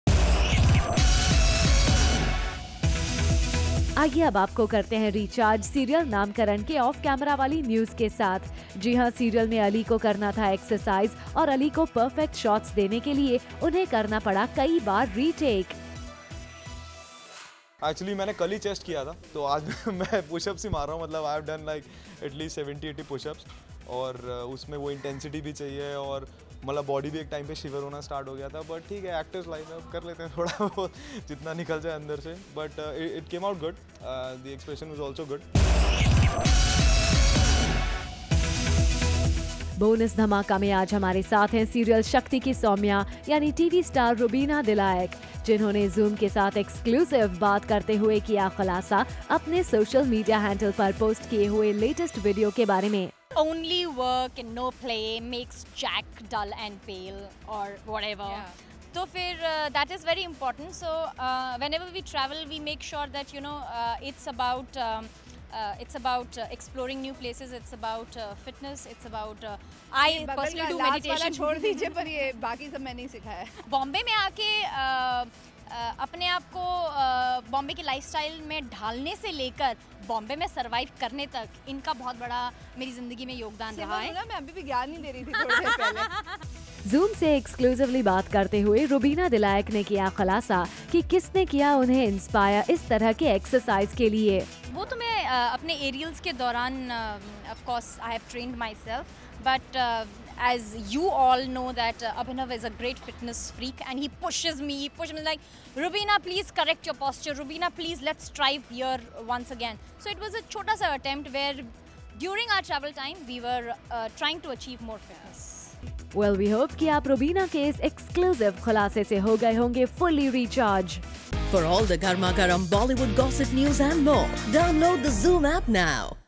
Ali’s Hard Work For Perfect Shot In Naamkarann | Rubina Talks About Her Motivation In Life
►Gautam Vig AKA Ali of Naamkarann talks about is hard work for one perfect shot.

►Rubina Dilaik AKA Soumya of Shakti talks about her fitness inspiration.